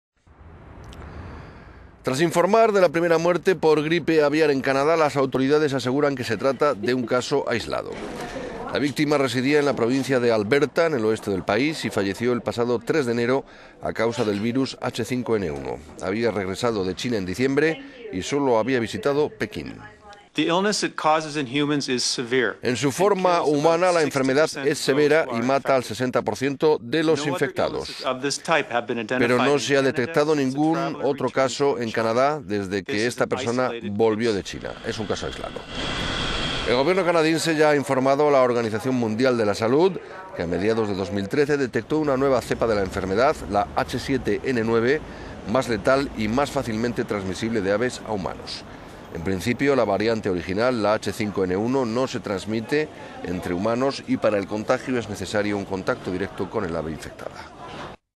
Primera víctima de la gripe aviar en Canadá
Tras informar de la primera muerte por gripe aviar en Canadá, las autoridades aseguran que se...

Tras informar de la primera muerte por gripe aviar en Canadá, las autoridades aseguran que se trata de un caso aislado. La víctima residía en la provincia de Alberta, en el oeste del país, y falleció el pasado 3 de enero a causa del virus H5N1. Había regresado de China en diciembre y solo había visitado Pekín.

Dr. Gregory Taylor, de la Seguridad Social canadiense:

" En su forma humana la enfermedad es severa y mata al 60 por ciento de los infectados. Pero no se ha detectado ningún caso más en Canadá desde que esta persona volvió de China. Es un caso aislado"